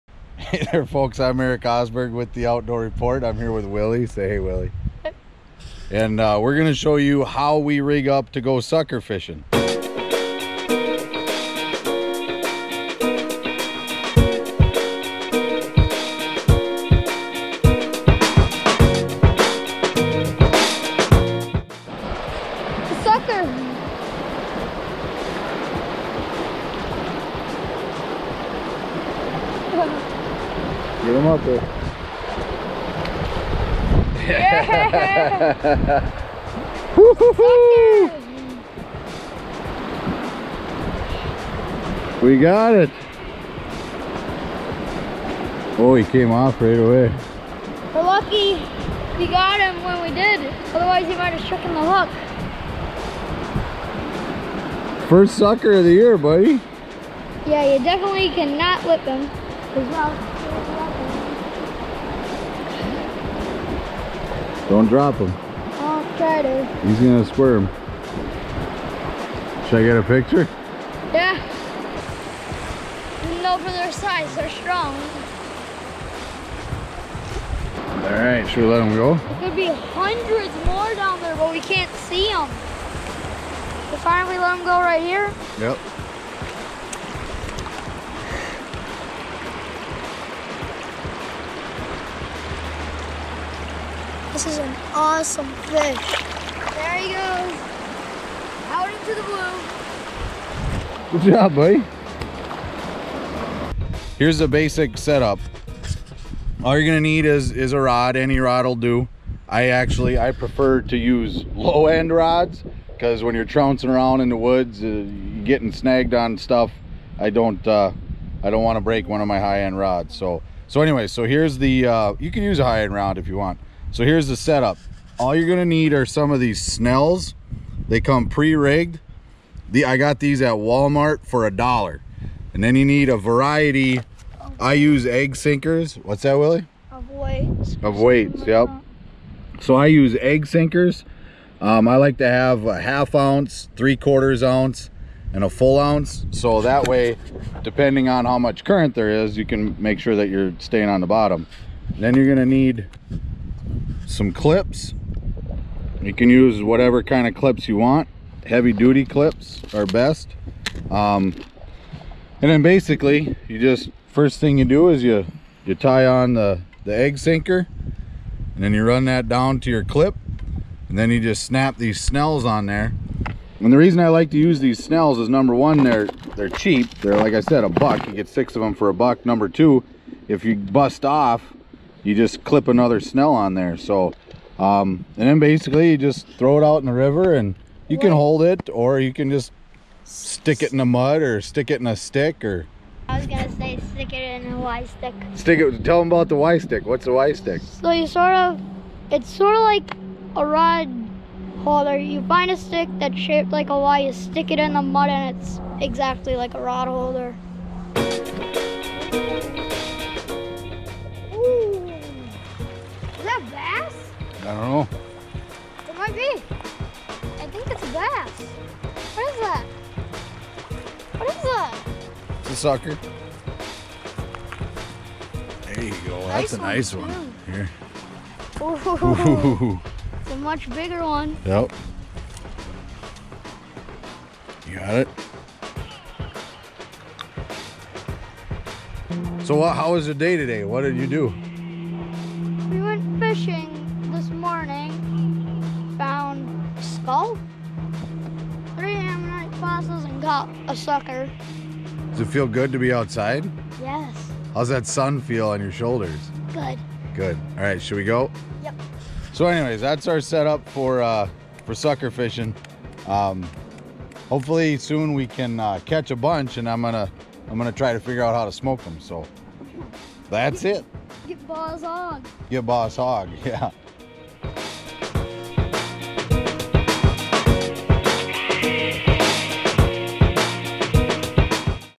How to catch suckers
We finally got to fish for suckers here in Minnesota this year. It wasn't crazy good, but it sure felt good. Here's a look at how it went & how we rig up.